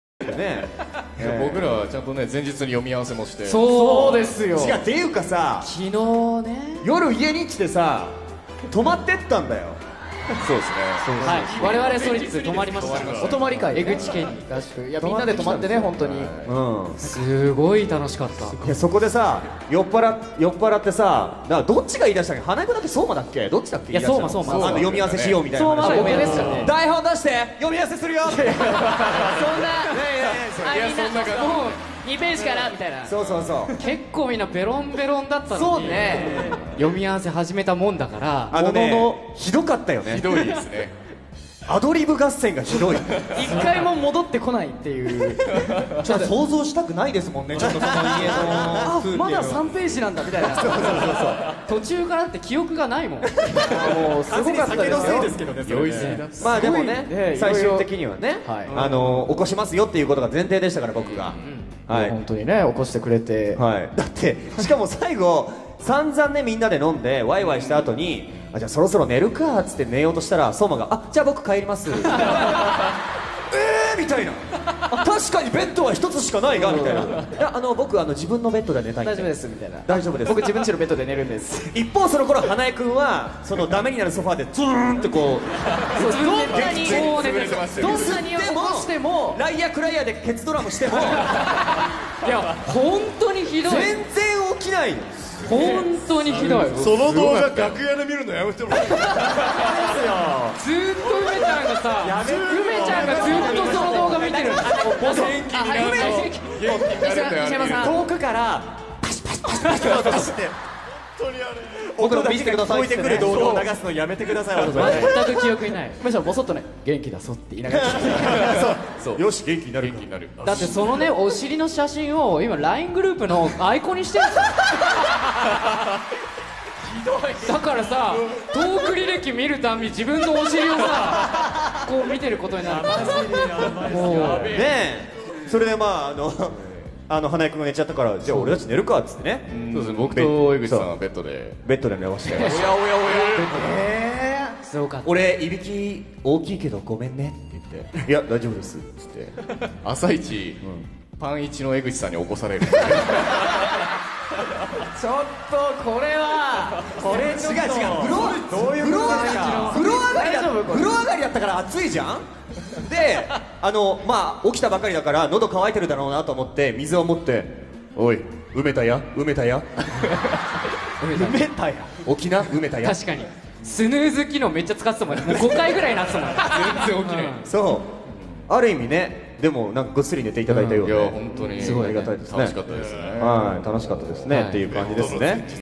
SQ / SolidS & Quell PARTY - MC
MC by SolidS and Quell Seiyuus

SolidS :

Eguchi Takuya as Takamura Shiki
Saitou Souma as Okui Tsubasa
Hanae Natsuki as Sera Rikka
Umehara Yuuichirou as Murase Dai

Quell :

Takeuchi Shunsuke as Izumi Shuu
Nishiyama Koutarou as Horimiya Eichi
Nakamura Shuugo as Kuga Issei
Nogami Shou as Kuga Ichiru